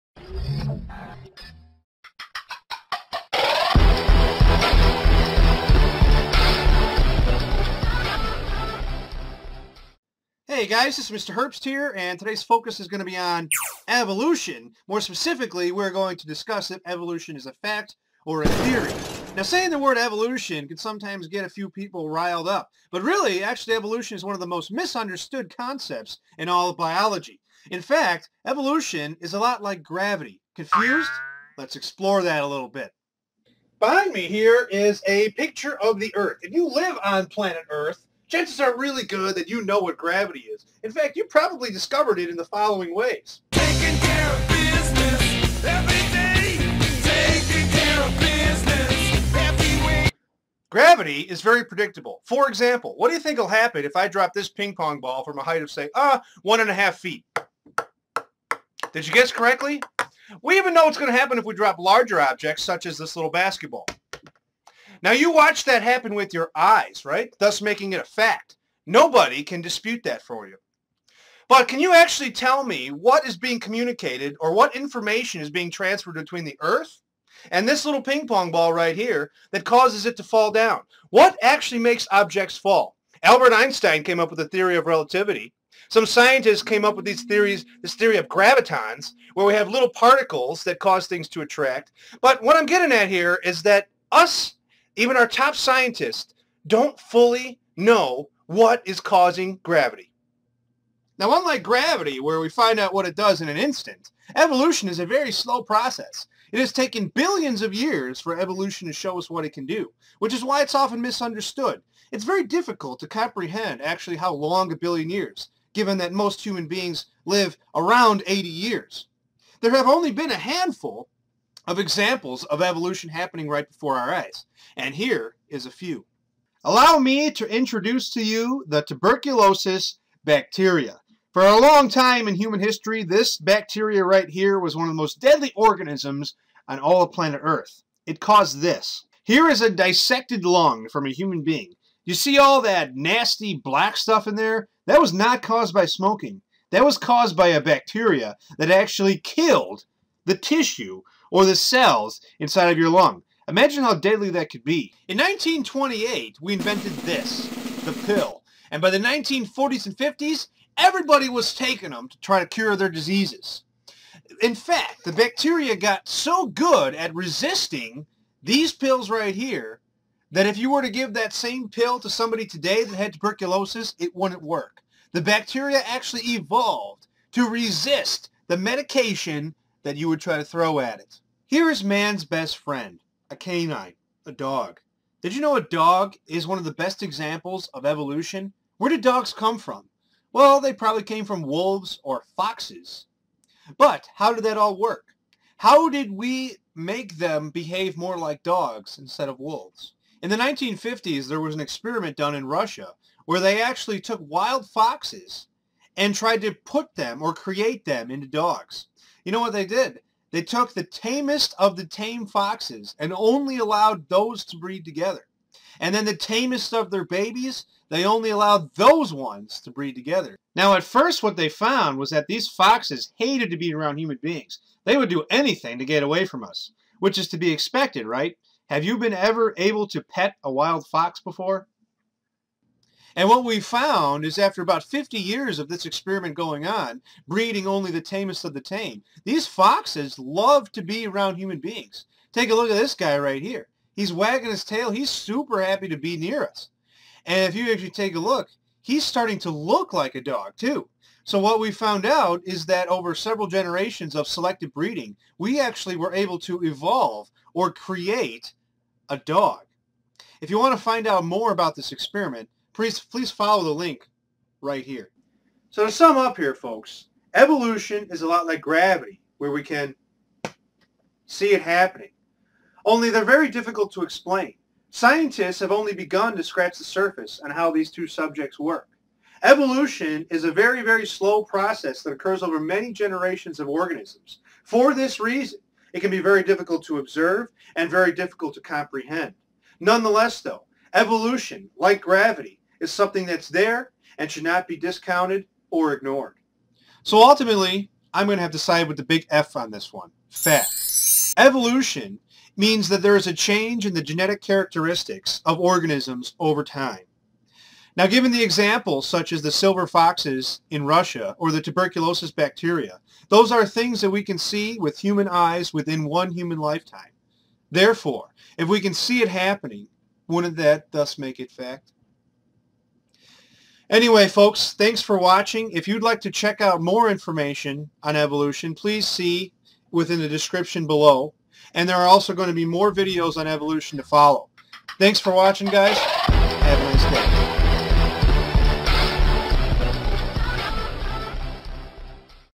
Evolution is like Gravity
This video compares evolution to gravity. Both can be seen right before our eyes yet they are both very difficult to explain.